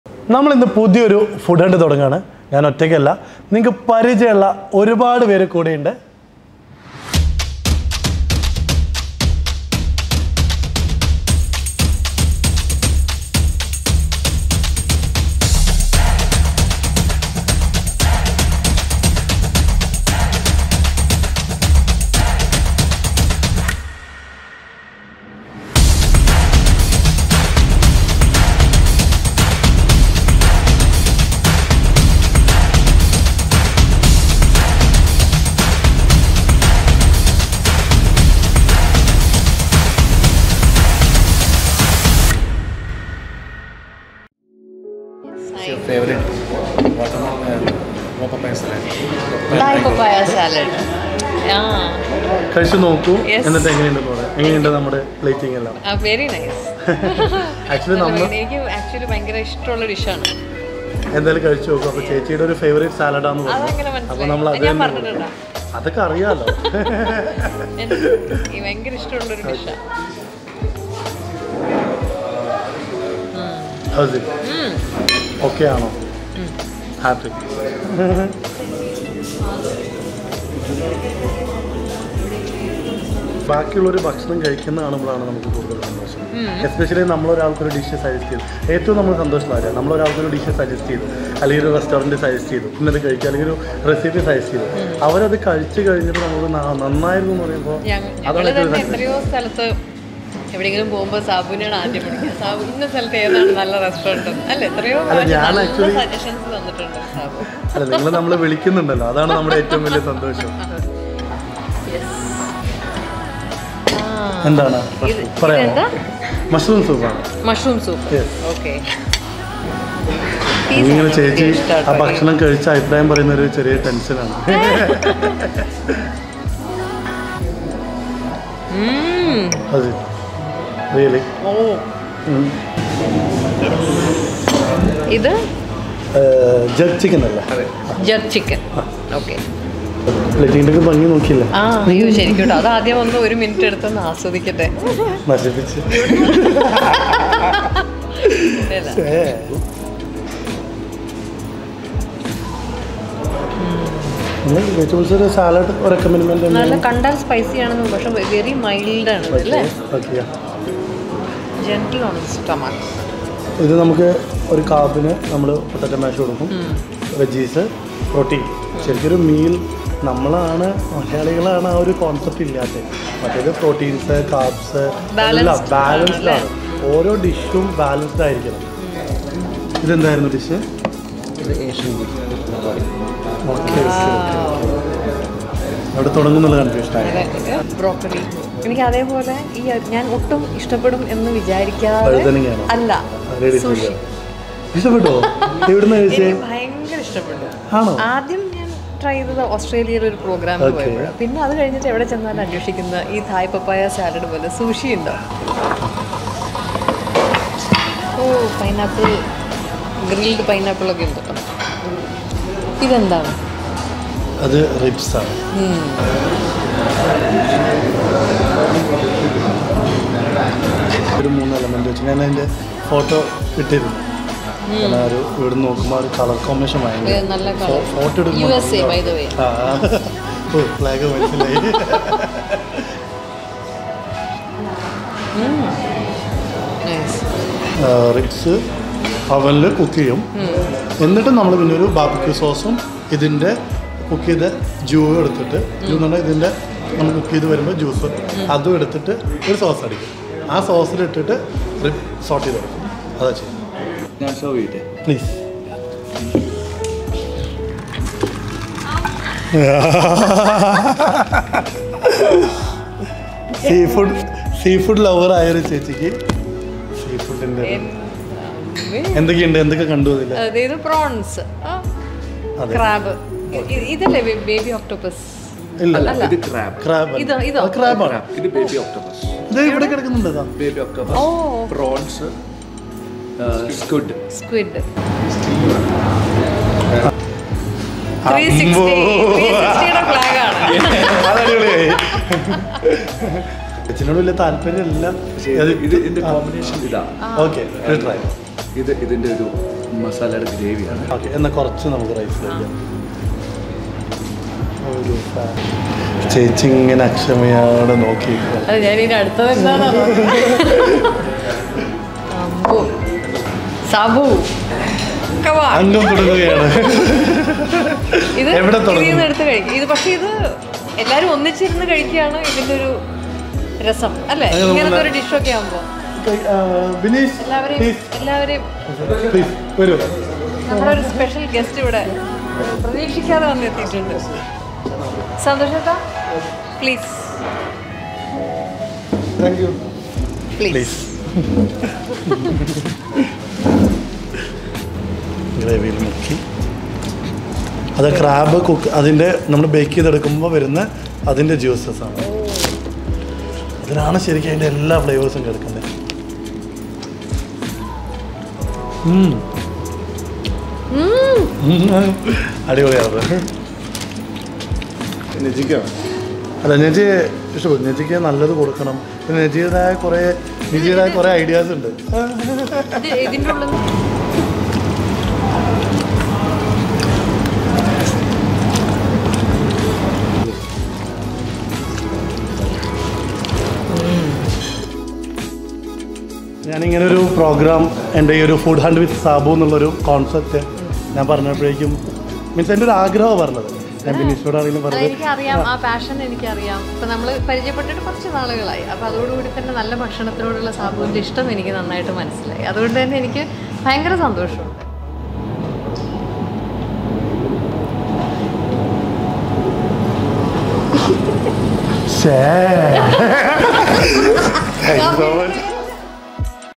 Food Hunt with Sabu | Featuring Manju Warrier | Episode: 01
Food Hunt with Sabu എന്ന നമ്മുടെ പുതിയ സീരീസിൽ ആദ്യ Episode കോട്ടയം 12 to 12 360 യിലെ ഭക്ഷണ വിശേഷങ്ങളുമായി മലയാള സിനിമയിലെ  നമ്മുടെ സൂപ്പർസ്റ്റാർ  മഞ്ചു വാര്യരോടൊപ്പമാണ്. വീഡിയൊ ഇഷ്ടമായാൽ അഭിപ്രായം കമന്റ് ചെയ്യാൻ മറക്കണ്ട.